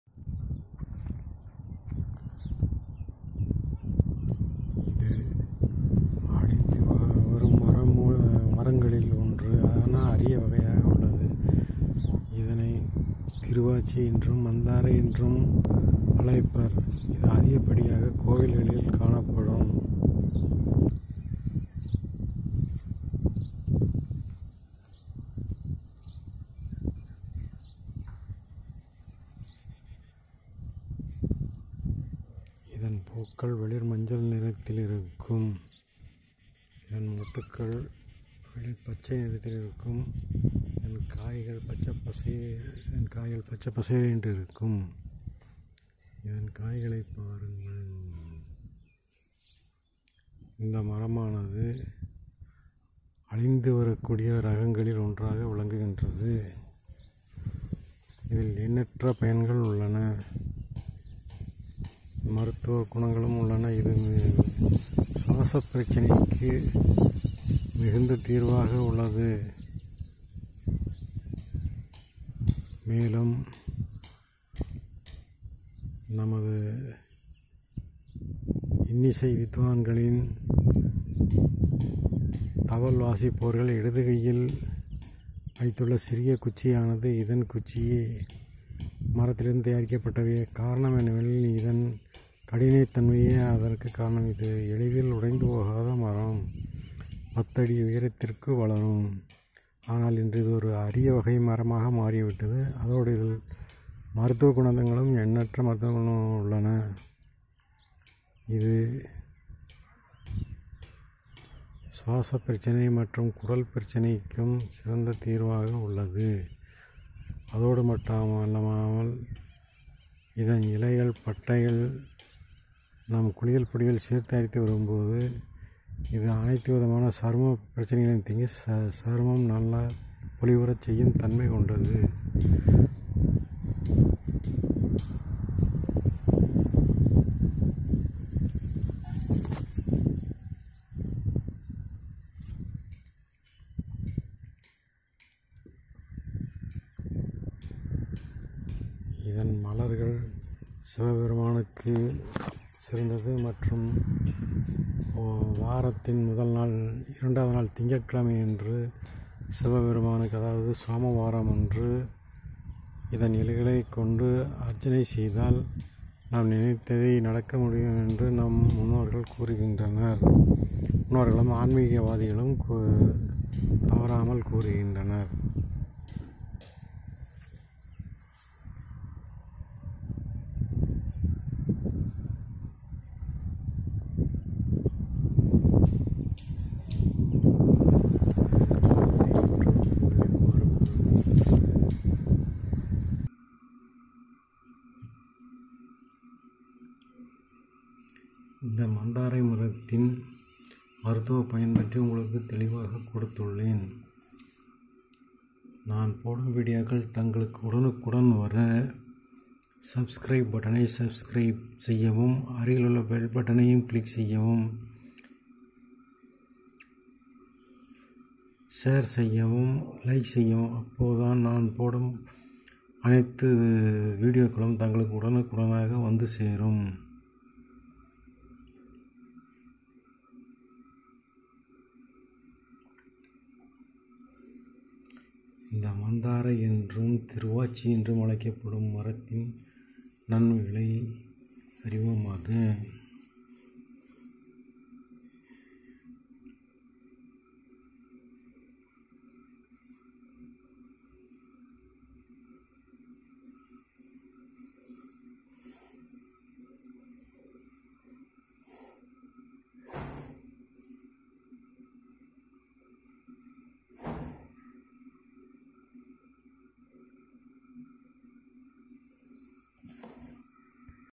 Vanishing Tree=Mantharai or Thiruvachi. No =24. seeds available.
This is a very good tree with huge medicinal properties. It is almost found in Sivalayams. The leaf looks like our respiratory system. It gives us glowing skin. It is very good remedy for respiratory problems. Apart from this the Stick in left hand of Mathalam musicians is made from Mantharai wood. So at least everyone plant a tree in our home. For seeds contact my wattsup number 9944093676